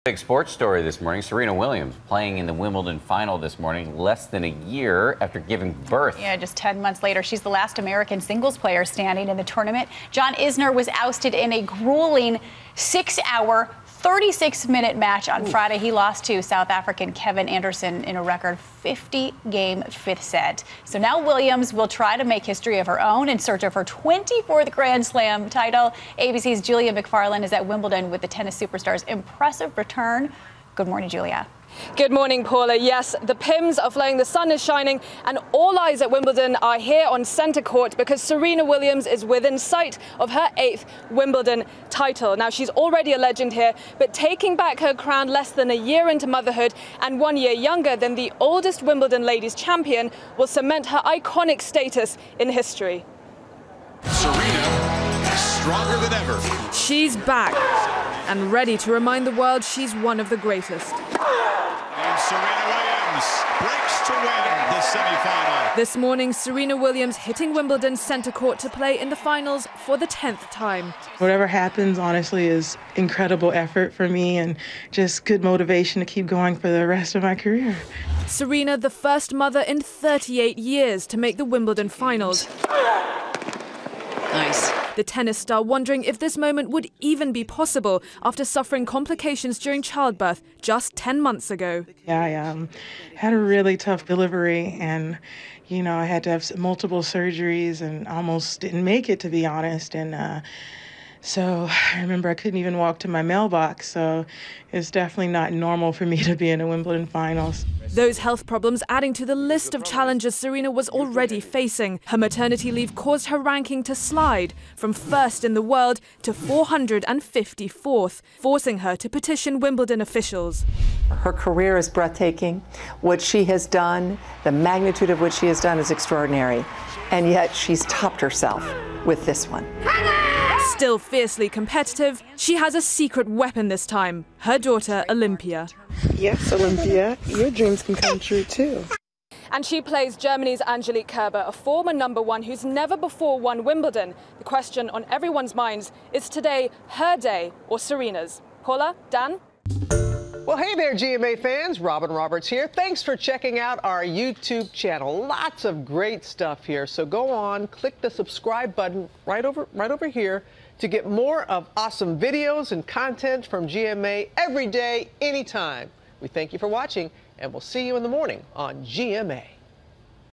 Serena Williams returns to Wimbledon
Williams will play in her 24th Grand Slam championship as the last American singles player in the tournament; Princess Kate and Duchess Meghan have arrived at Wimbledon to cheer her on.